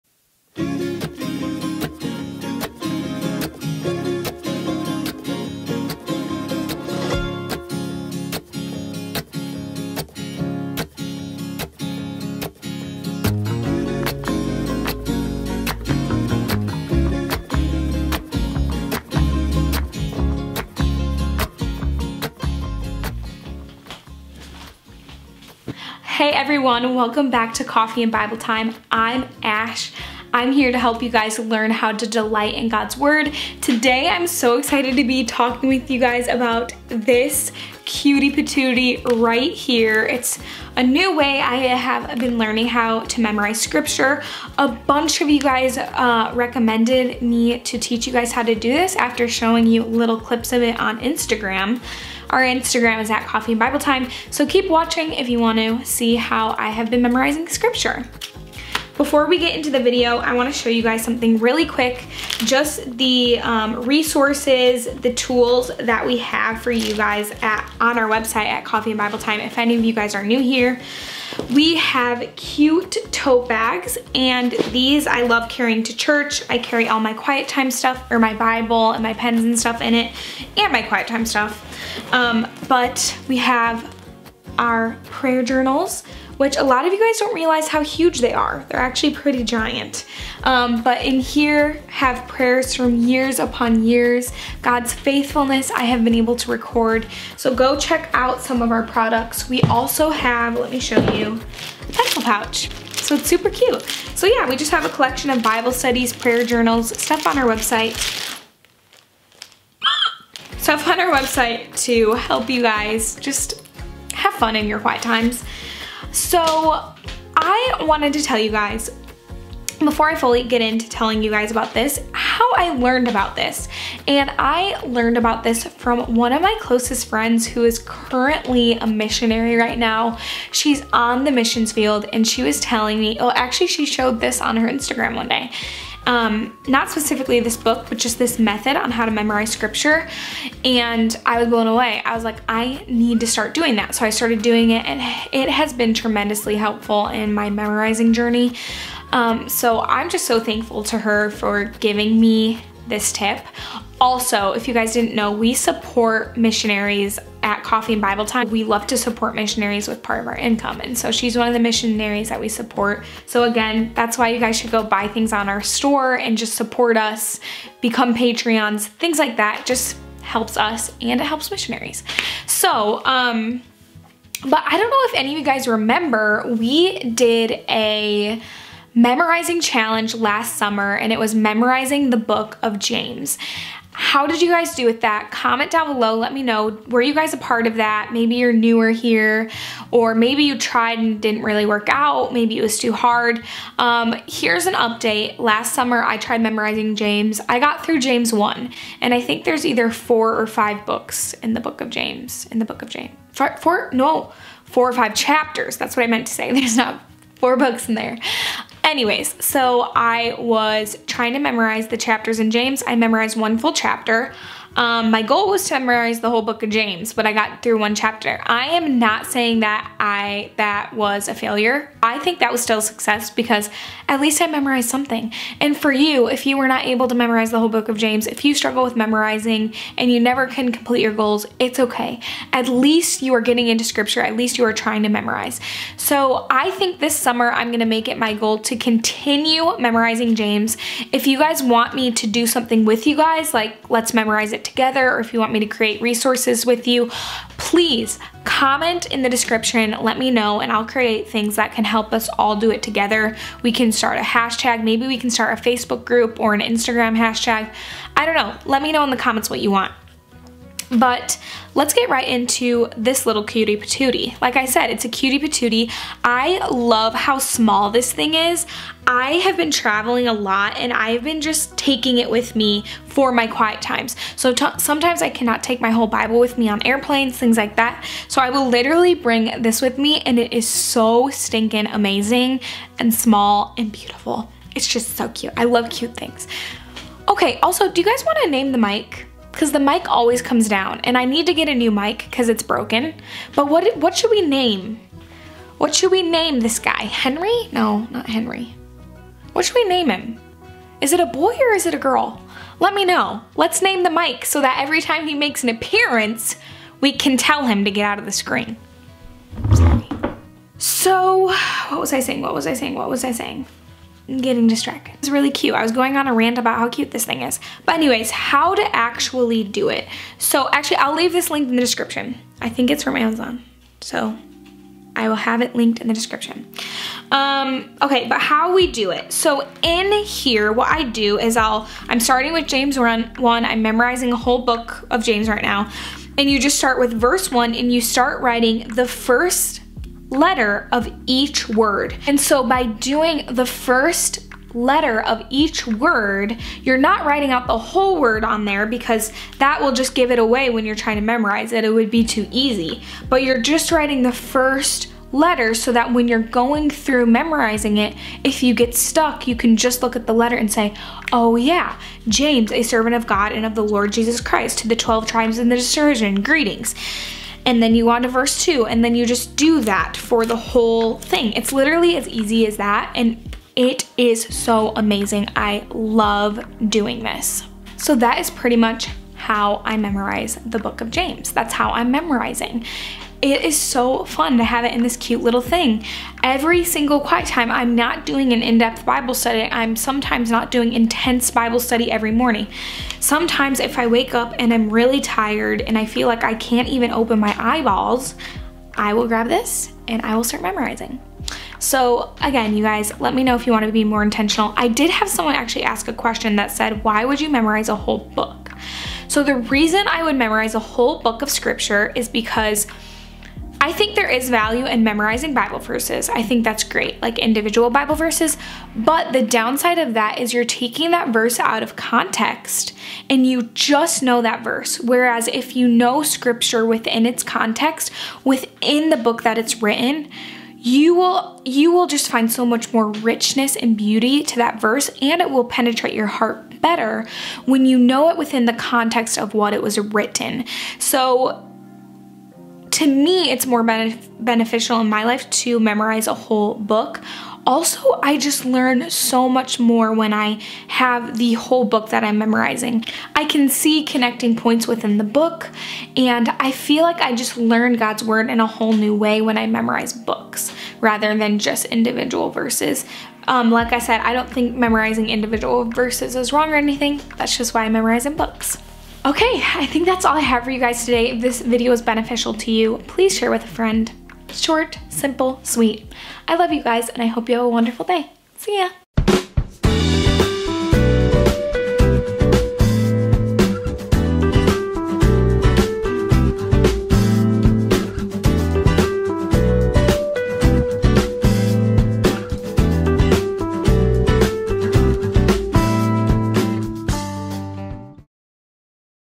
HOW I MEMORIZE SCRIPTURE - memorize whole books!
Hey Friends! Here is how I have been memorizing scripture lately... AND IT HAS BEEN A GAME-CHANGER! I cannot take credit for this method - I learned it from a good friend of mine who is a missionary! She inspires me and is DEDICATED to storing scripture in her heart!
I have found that memorizing scripture is LIFE-CHANGING because it is storing God's LIVING AND ACTIVE Word in my heart - and when it is in my heart, I have it ALWAYS to turn to. If you memorize Scripture, the Holy Spirit will bring up God's word throughout the day to sharpen, correct, train, encourage, and comfort you. MEMORIZING SCRIPTURE should be one of our highest priorities and disciplines as we grow spiritually!

MY NEW BIBLE THAT I LOVE

Vlog Camera

Coffee Kettle

Coffee Dripper

Coffee we LOVE

AMAZON STORE: